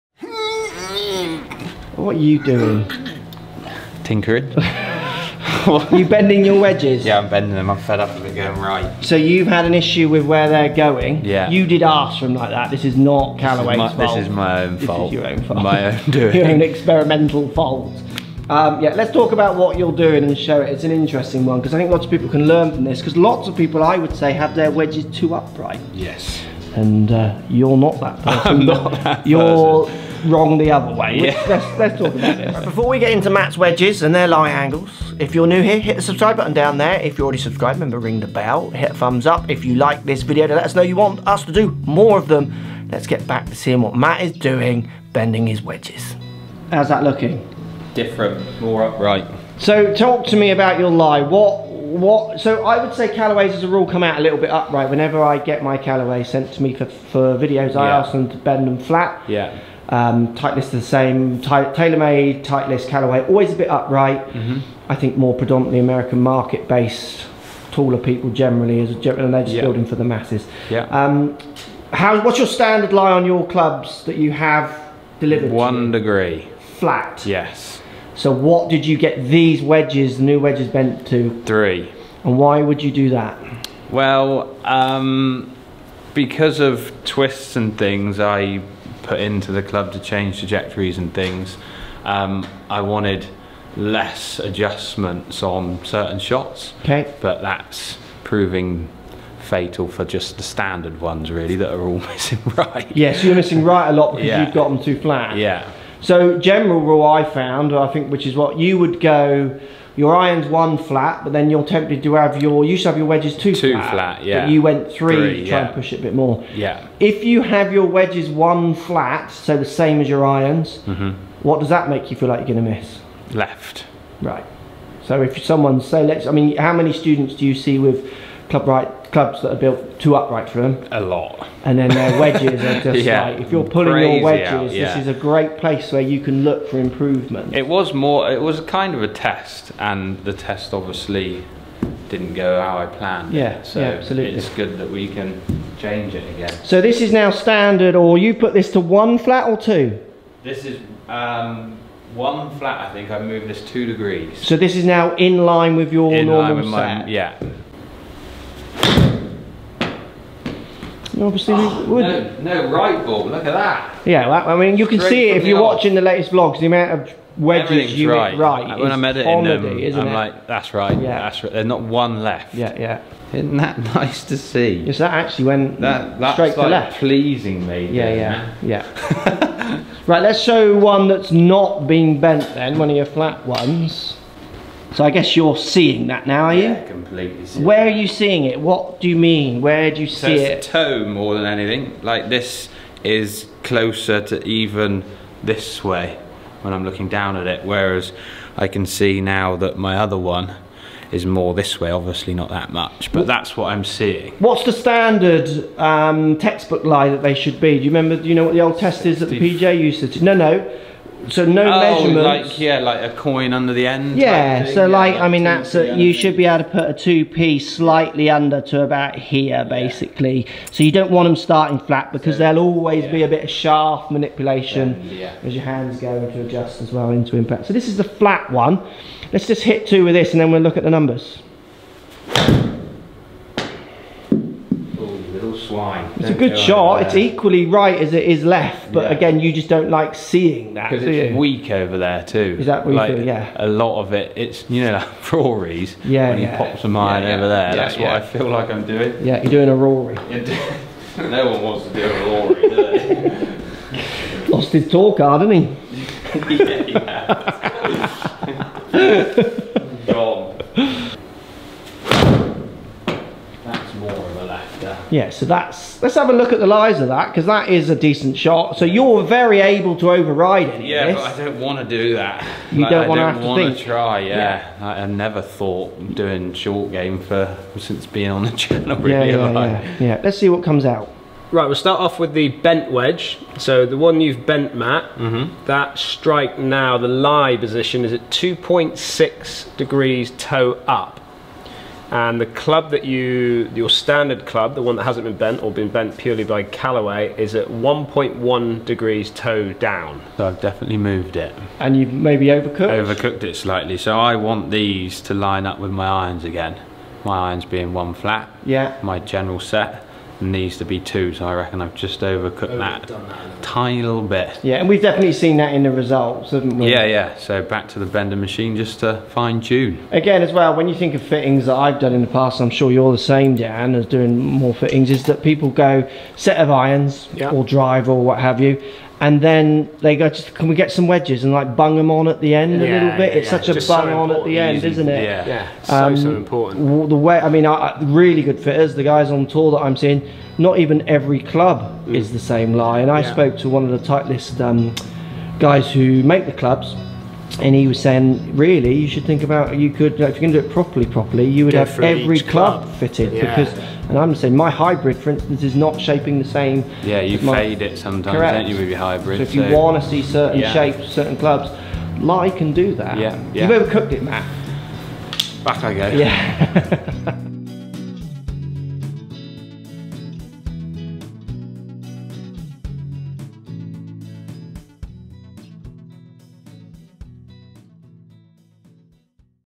GOLF CLUB FITTING BENDING CLUBS
Golf club fitting and bending clubs with Mark Crossfield and friends. Getting your golf clubs fitted and bending lies and lofts to the spot you need them to play your best golf is an important part of golf coaching and golf club fitting. Coach Lockey bends his wedges to try and get his golf game and golf short game back where he wants it and to help him get ups and down more often. Custom fitting golf clubs can use facts, delivery numbers but it has to work with the golfer to make sure what they want to see what they want to feel and what they play works for them on the golf course.